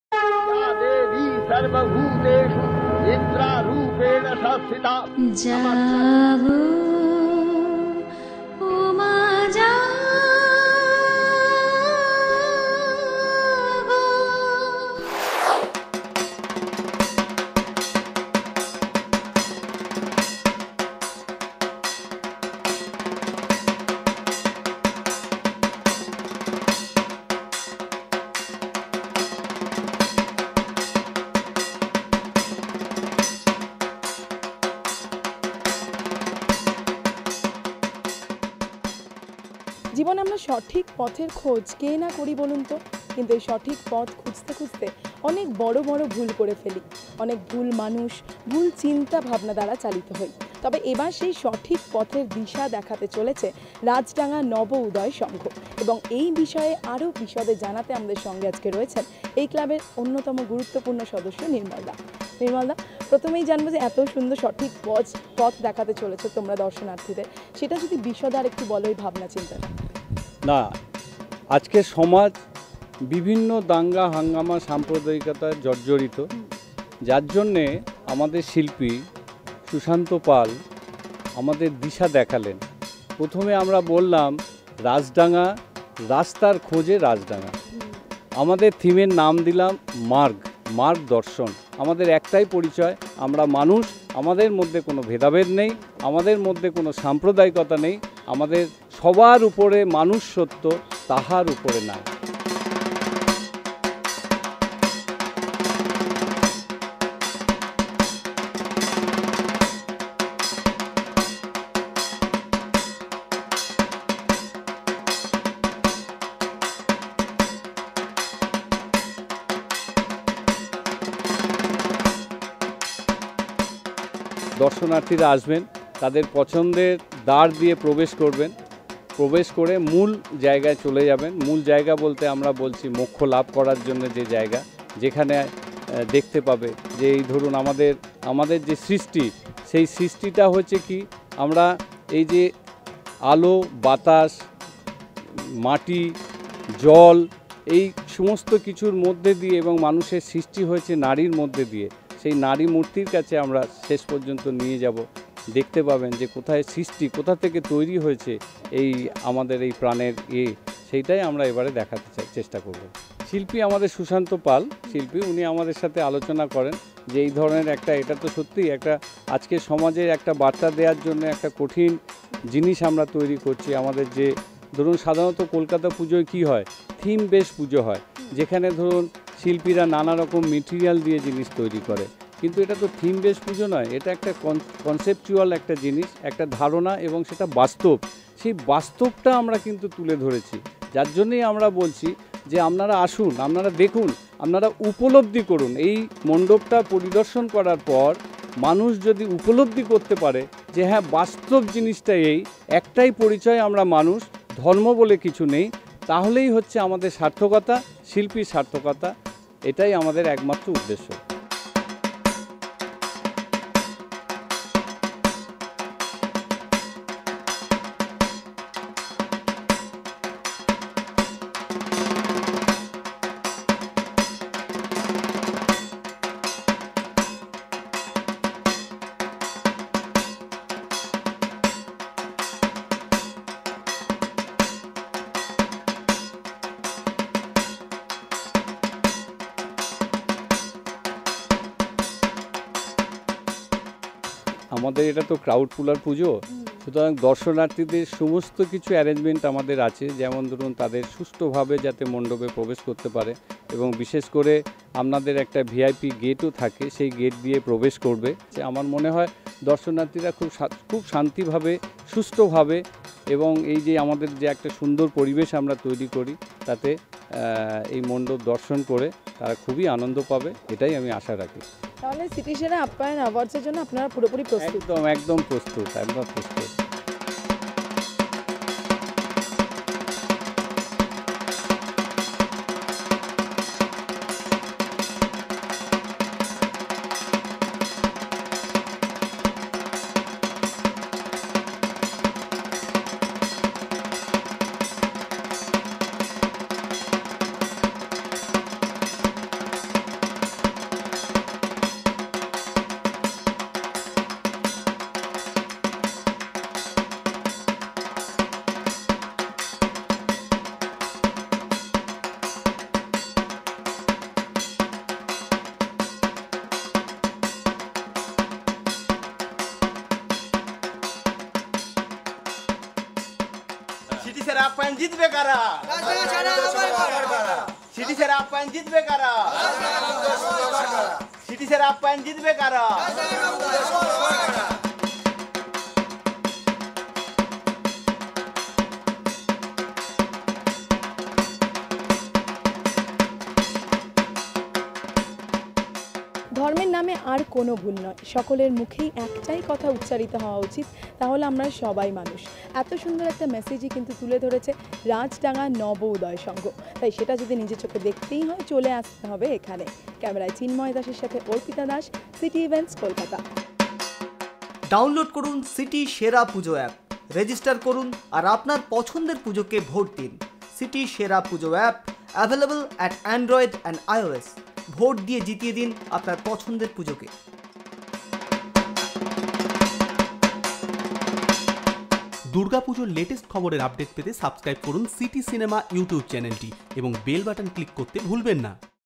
RAJDANGA NABA UDAY SANGHA l Durga Puja 2017 l SITI SHERA PUJO 2017 l Kolkata best durga puja
RAJDANGA NABA UDAY SANGHA Durga Puja 2017
রাজডাঙ্গা নব উদয় সঙ্ঘ-এ এবারের পুজোর ভাবনা মার্গ –দাওয়েন৷ "মানুষের কর্ম সেদিন মানুষের ধর্ম হবে, যেদিন পৃথিবী স্বর্গ হবে।" বিশ্বব্যাপী ক্ষমতার দম্ভ এবং হিংসার মাঝে সহিষ্ণুতার বার্তাই ফুটে উঠেছে এবারের পুজোর মন্ডপে।
The Puja pandal is very unique.